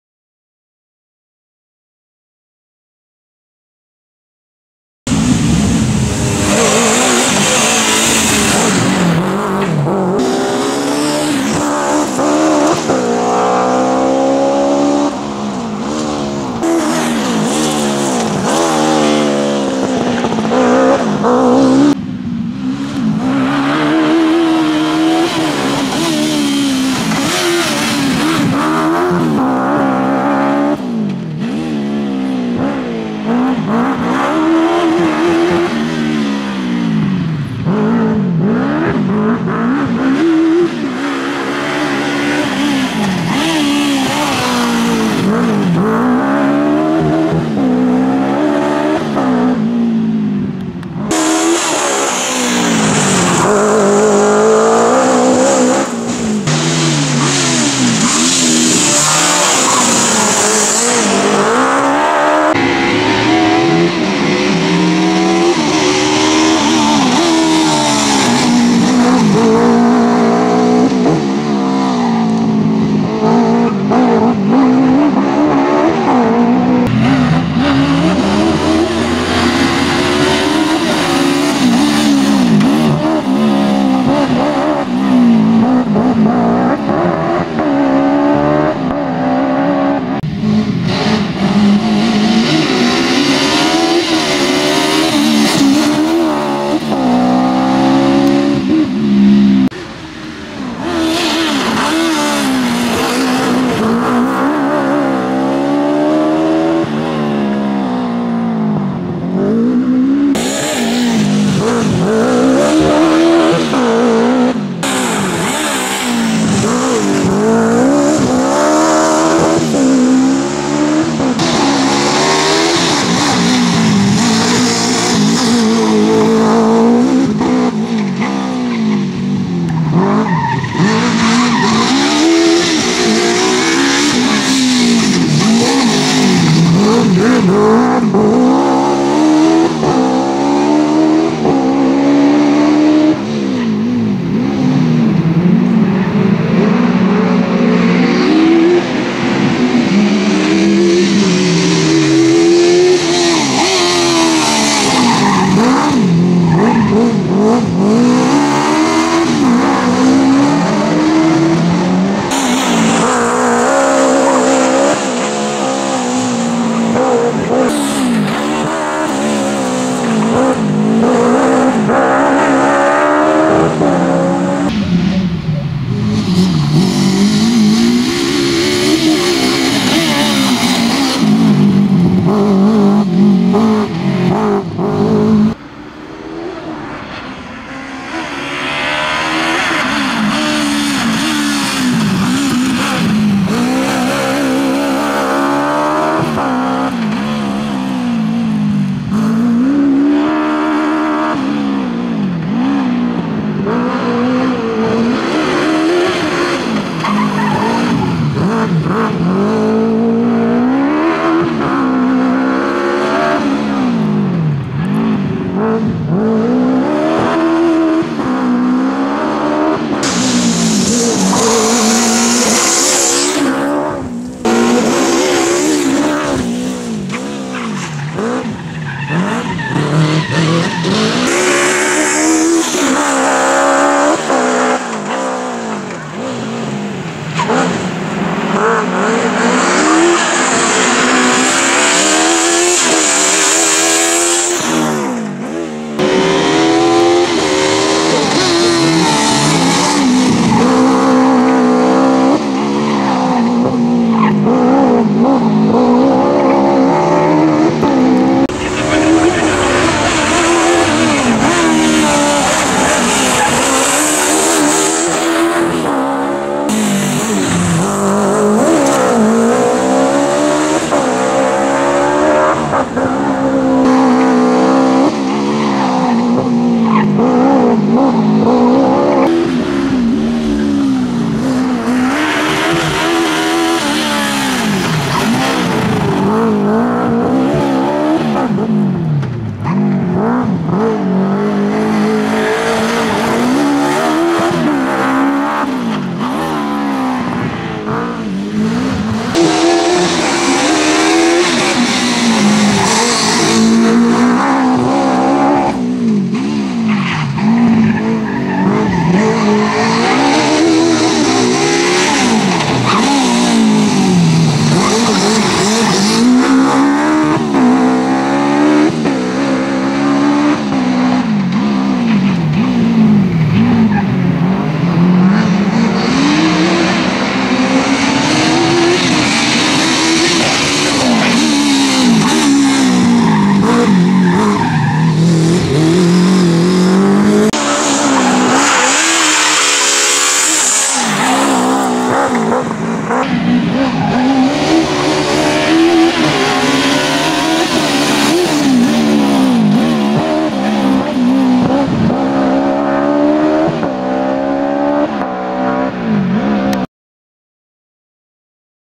Szombierki Rally Cup 2021 - Runda 3 - Hus / Hus - Suzuki Swift Proto
Szombierki Rally Cup 2021 - Runda 3 - Sebastian Hus / Justyna Hus - Suzuki Swift Proto